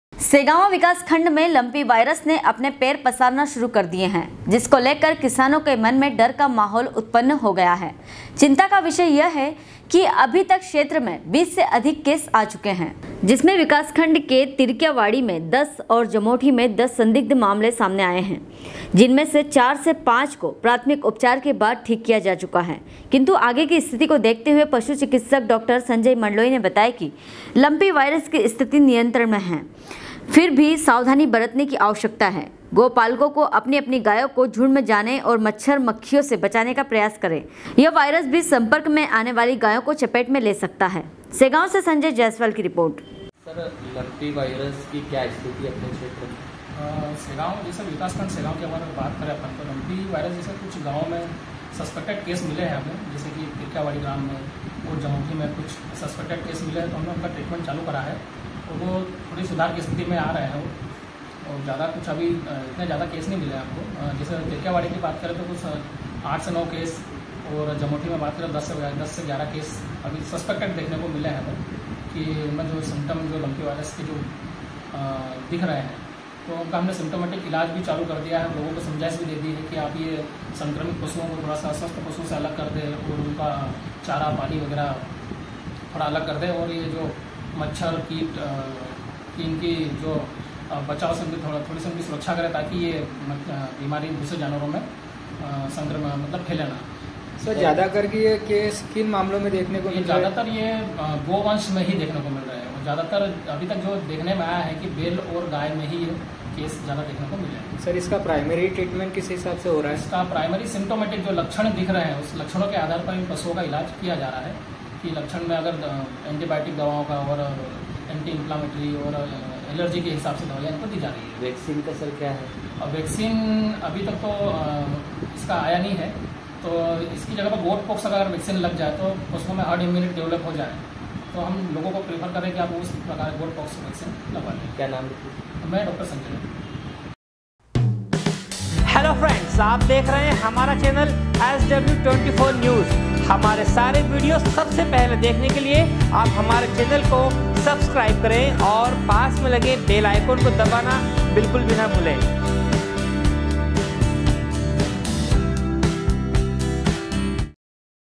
सेगांव विकास खंड में लंपि वायरस ने अपने पैर पसारने शुरू कर दिये है जिसको लेकर डर का माहौल उत्पन
सेगांव विकास खंड में लंपि वायरस ने अपने पैर पसारने शुरू कर दिये है जिसको लेकर किसानो के मन मे डर का माहौल उत्पन हो गया है चिंता का विषय यह है कि अभी तक क्षेत्र में 20 से अधिक केस आ चुके है सेगांव से संजय जायसवाल की रिपोर्ट